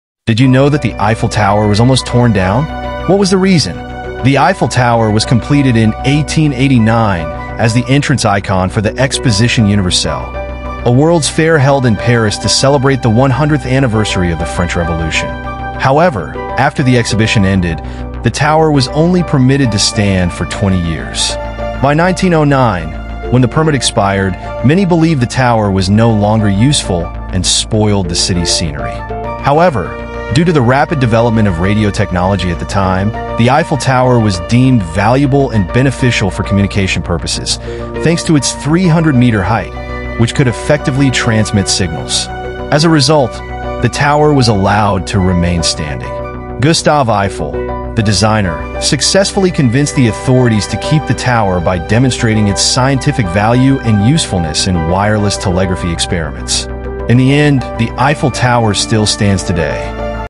Eiffel Tower: The Surprising Reason It Nearly Disappeared
Did you know the Eiffel Tower was almost demolished? Built in 1889 for the 'Exposition Universelle' celebrating the 100th anniversary of the French Revolution, the tower was only supposed to stand for 20 years. By 1909, many wanted it removed, believing it had outlived its usefulness. But thanks to the rise of radio technology and its importance for communication, the tower was saved and still stands today. Discover how Gustave Eiffel's vision kept this iconic structure alive!